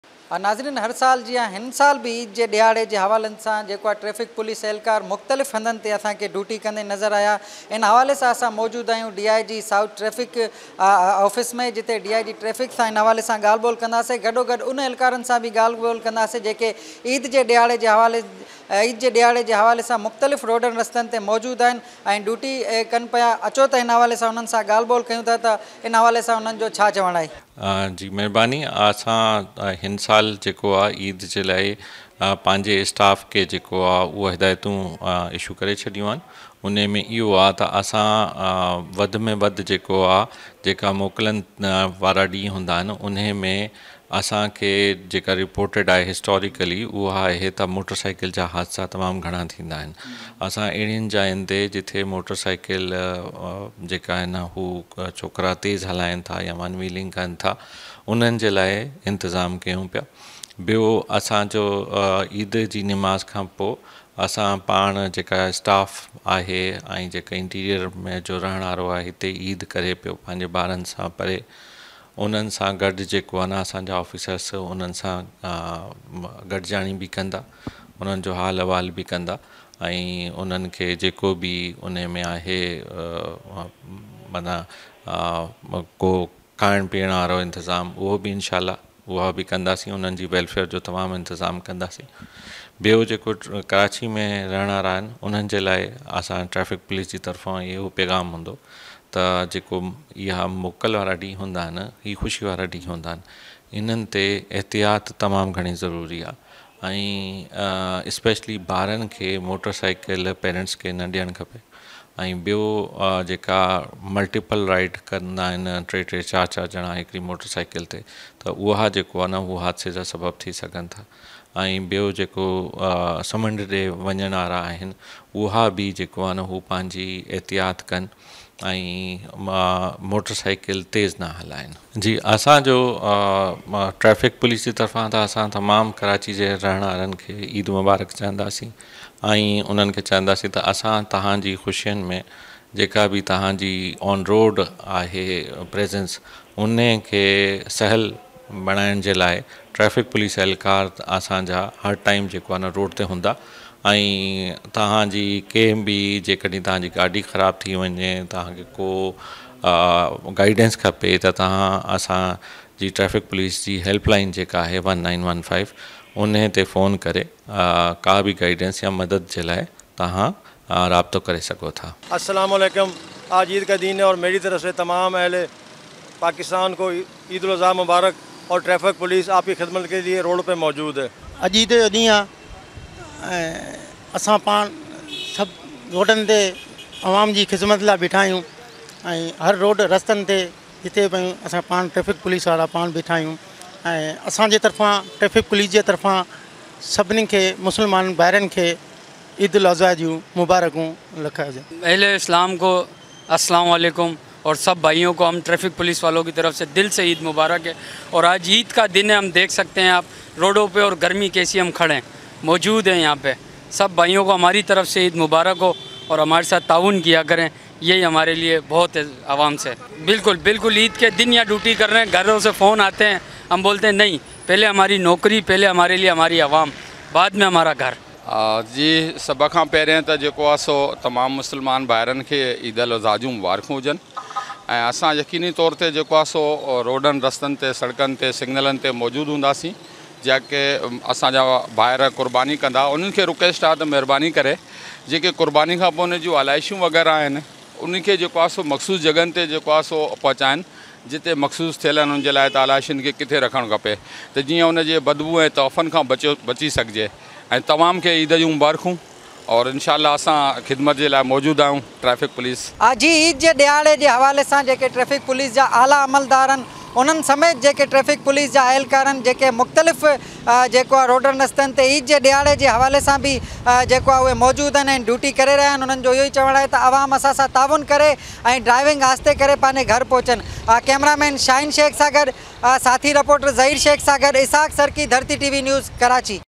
Karachi: Traffic Police Eid Diharan Main One-Wheeling Rokanr Lai Hikmat e Amli Joore Warti
ڪراچي: ٽرئفڪ پوليس عيد ڏهاڙن ۾ ون ويلنگ روڪڻ لاءِ حڪمت عملي جوڙي ورتي